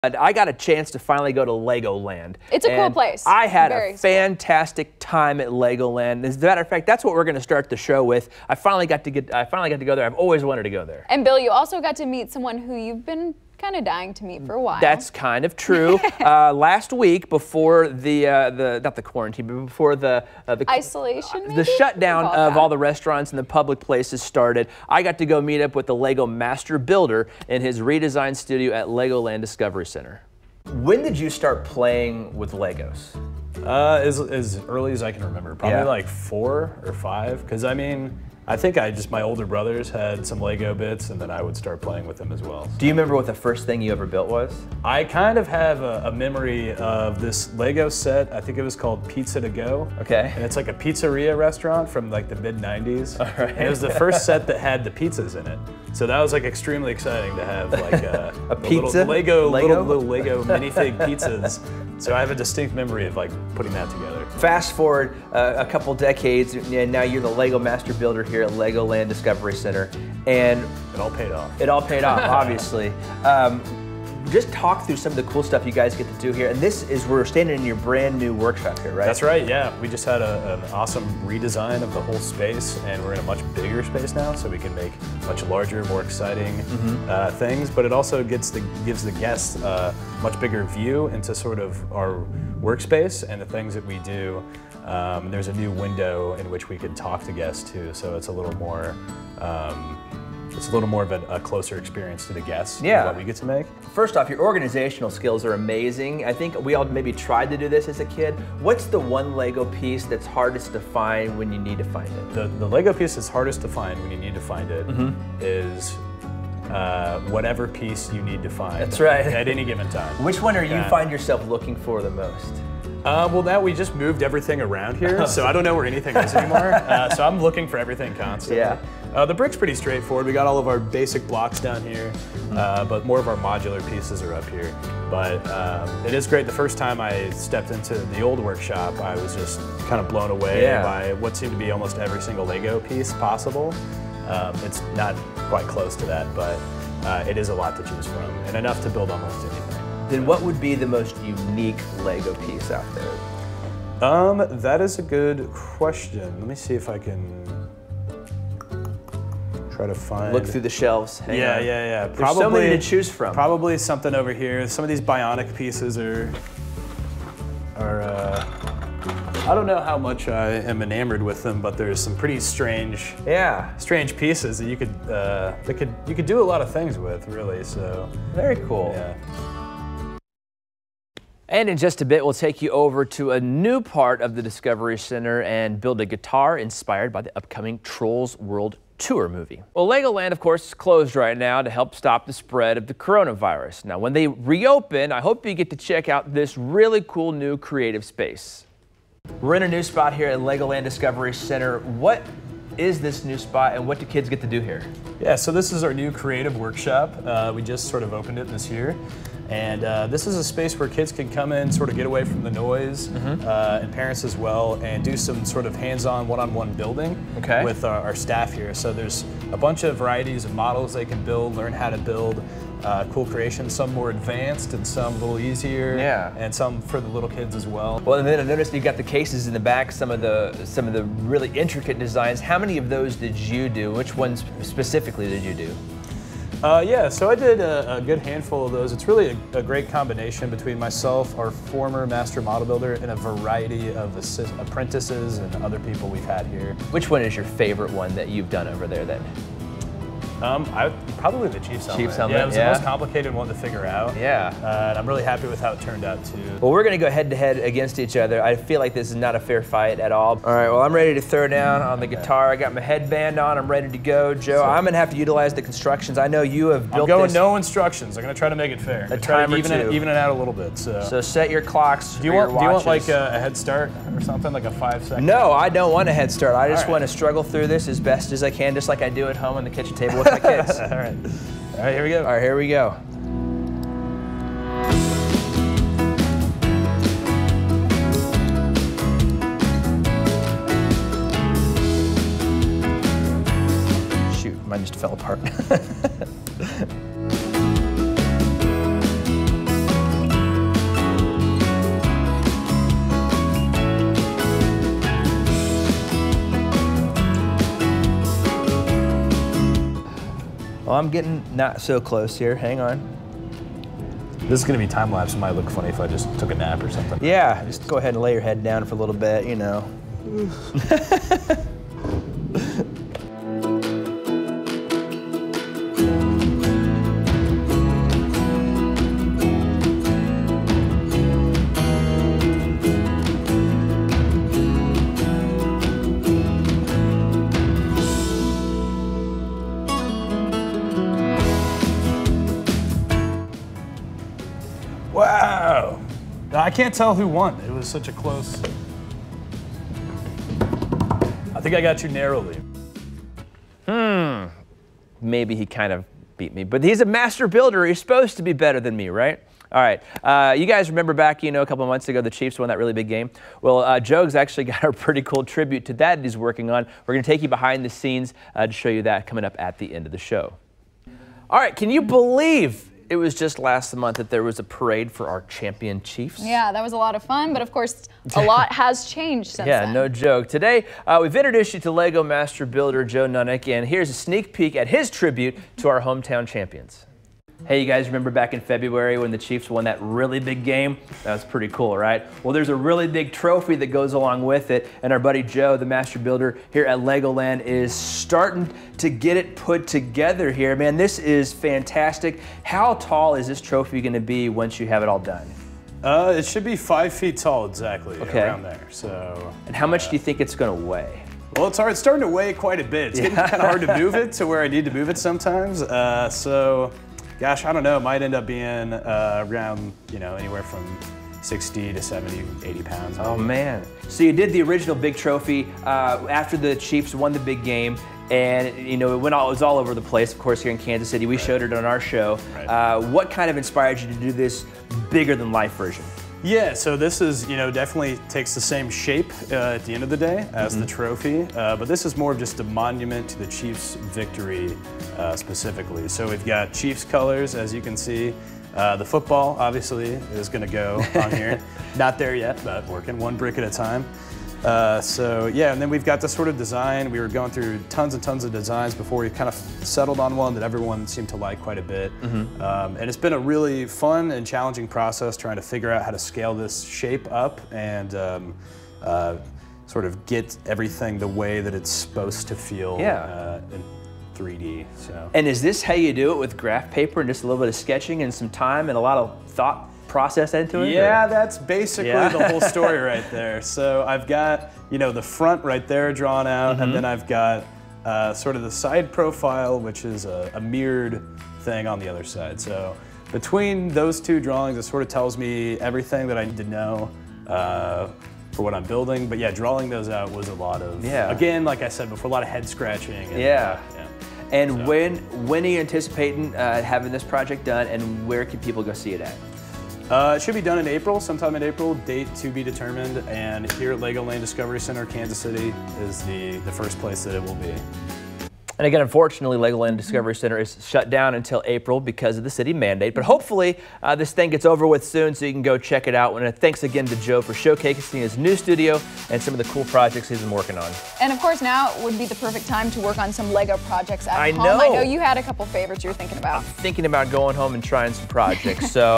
Meet Legoland master builder
Master model builder Joe Nunnink shows us around Legoland and explains what it takes to be a master model builder.